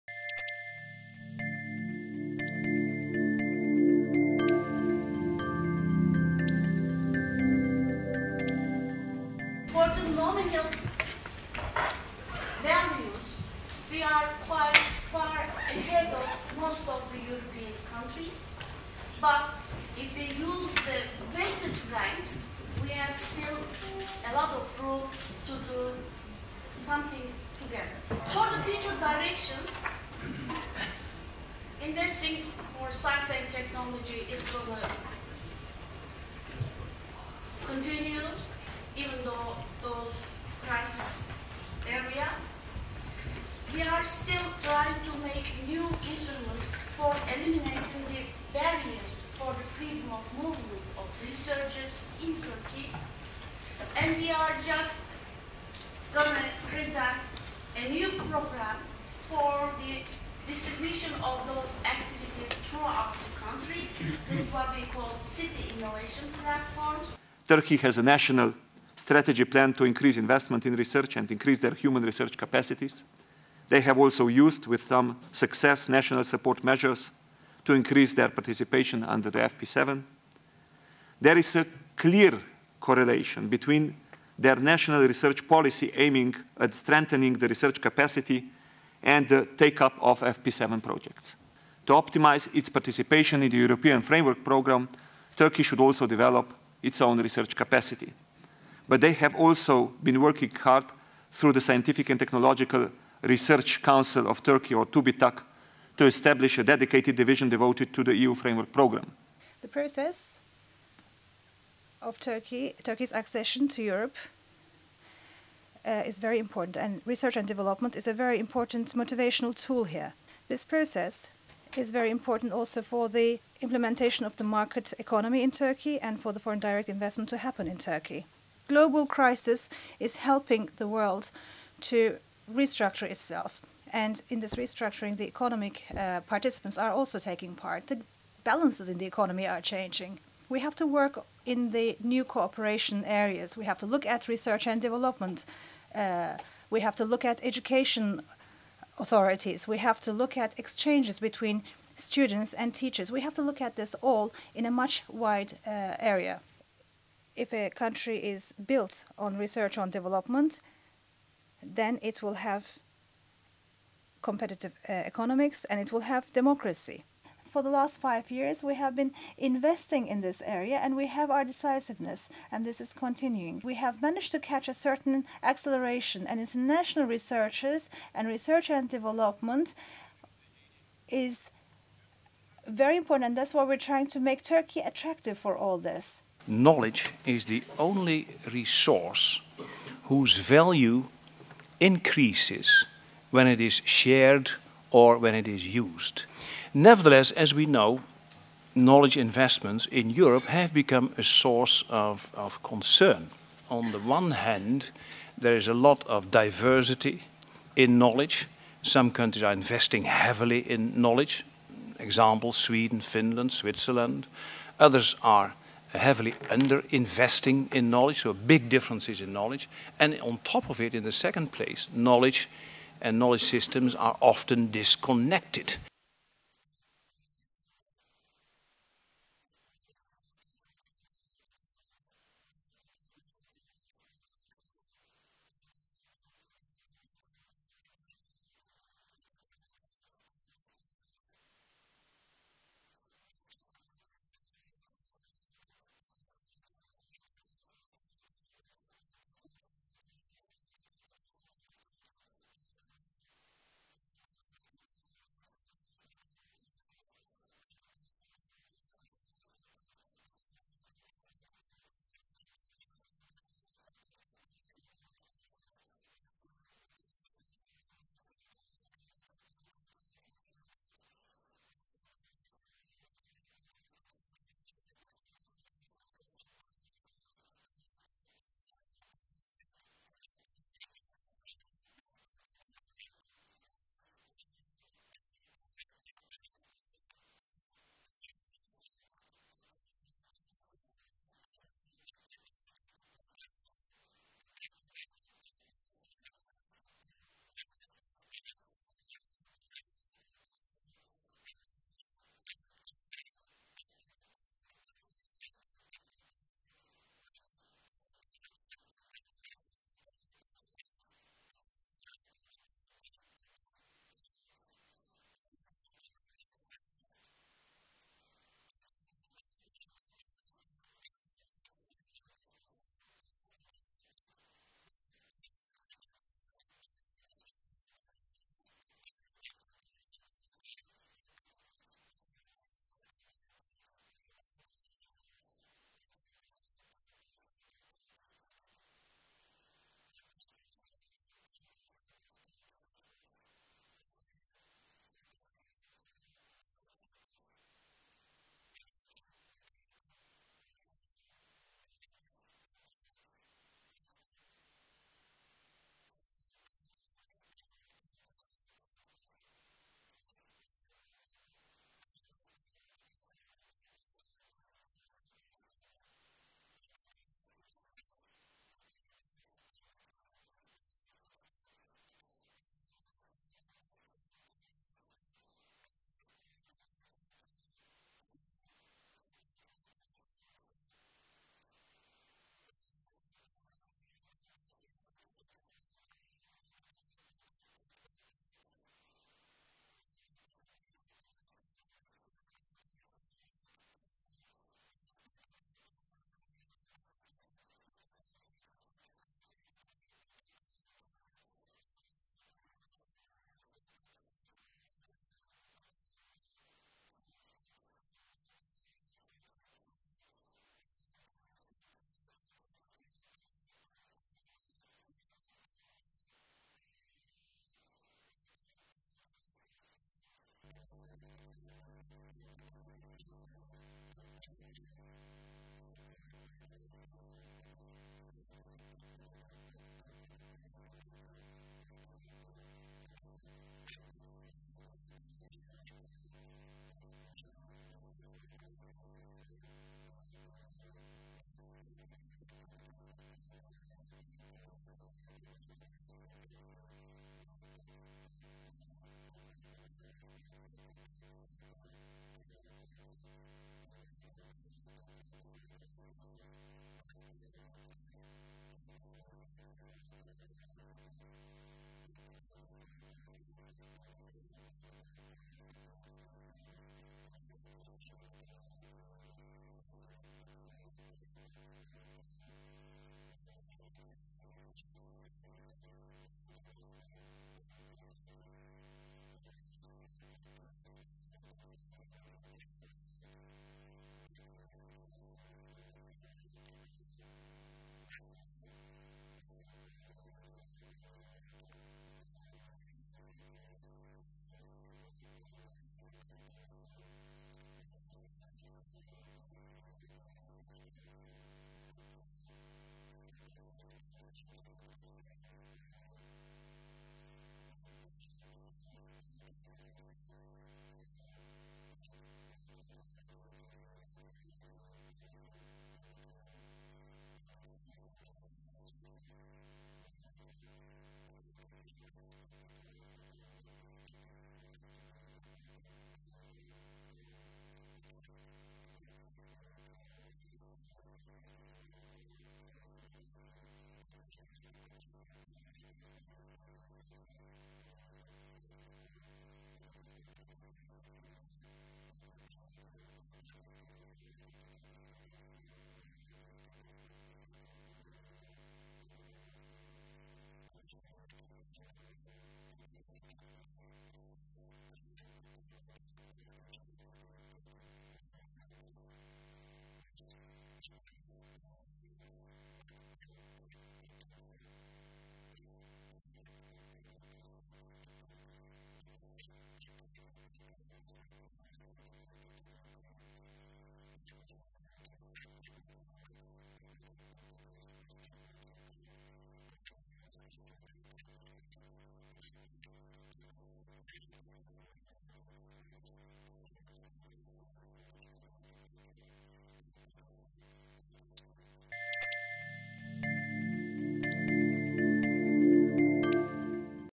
[ALEV] ALDE Seminar "Turkish R&D day..."
Video summary of the ALDE Seminar "Turkish R&D day - Boosting R&D excellence in times of economic downturn: insights from Turkey"

Event date: 29/01/09 09:00 to 12:30
Location: Room JAN 4Q1, European Parliament, Brussels

Alliance of Liberals and Democrats for Europe (ALDE) in the European Parliament together with Turkish Research and Business Organisations (TURBO)* is organizing Turkish Research & Development Day on January 29th.

With the theme of "Boosting R&D Excellence in Times of Economic Downturn: Insights from Turkey", Turkish R&D Day aims at increasing collaboration within the R&D and Innovation related European Programmes such as FP7, CIP and EUREKA. Turkish R&D Day will bring prominent politicians from Turkey and European Parliament, high level Turkish and EC officials coordinating the R&D and Innovation programmes, R&D directors of related international institutions and companies, and university rectors together.

MEP's Concerned:
Vittorio Prodi

[ALDEEVENTS] [Events] [Seminar] [Language EN]